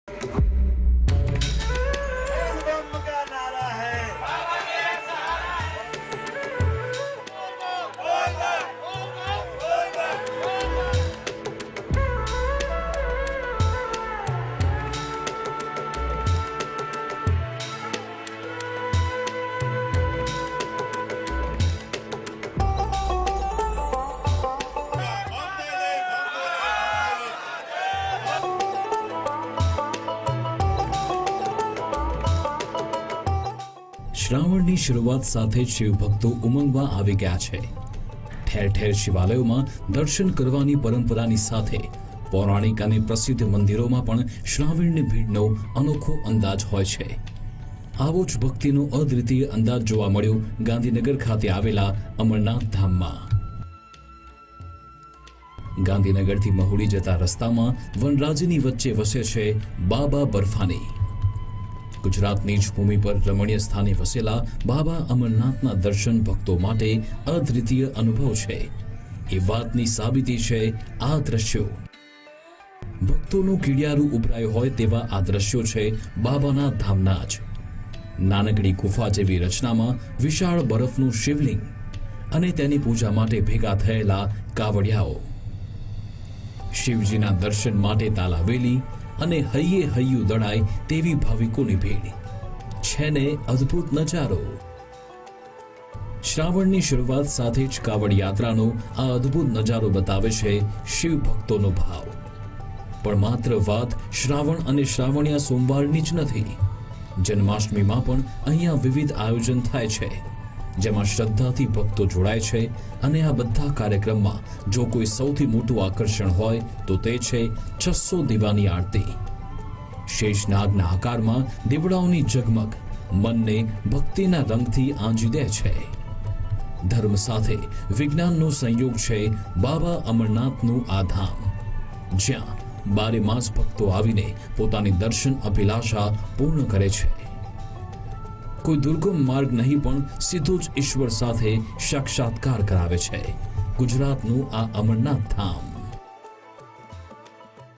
Shravan Month: 5 ફુટ ઉંચા બરફના શિવલિંગના અલભ્ય દર્શન
ગાંધીનગર મહુડી રોડ પર શિવસ્થાન
પ્રકૃતિના સાનિધ્યમાં શિવજીનો નિવાસ
પવિત્ર અમરનાથ ધામની મુલાકાત
5 ફુટ ઉંચા બરફના શિવલિંગના અલભ્ય દર્શન
વૈજ્ઞાનિક પદ્ધતિથી 365 દિવસ થાય દર્શન
55 કિલોમીટરની કાવડયાત્રાનું આયોજન